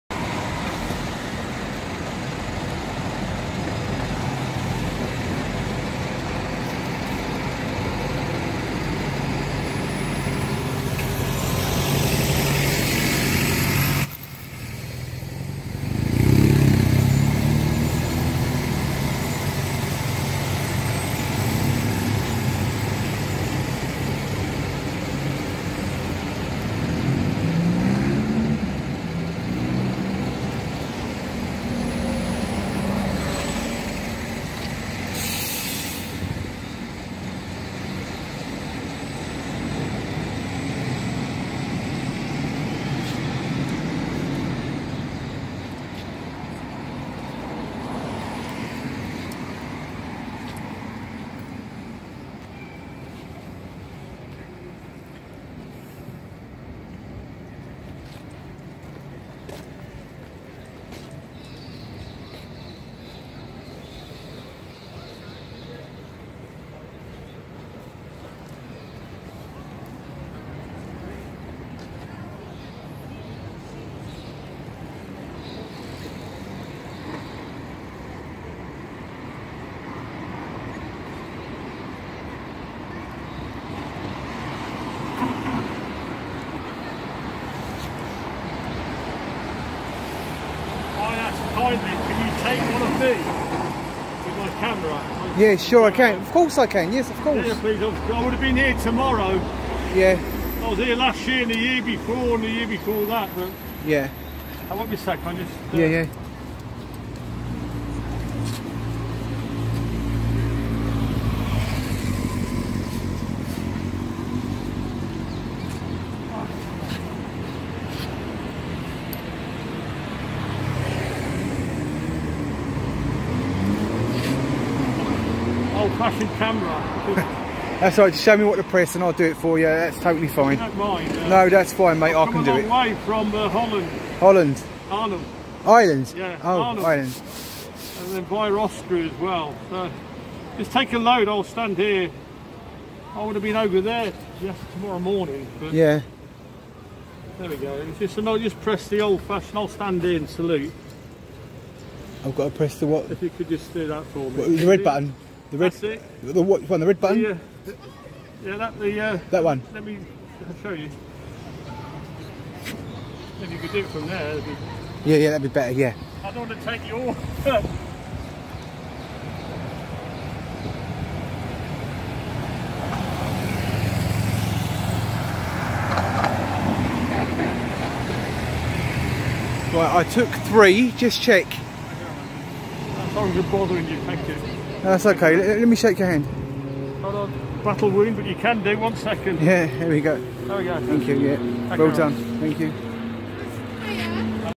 Soldier lays his hat at the The Cenotaph Lestweforget.
# WHITEHALL THECENOTAPH POPPYAPPEAL THIS SOLDIER COULD NOT MAKE IT ON SUNDAY MAKE BECAUSE OF THE COVID RULES HE ASKED ME TO TAKE HIS PICTURE.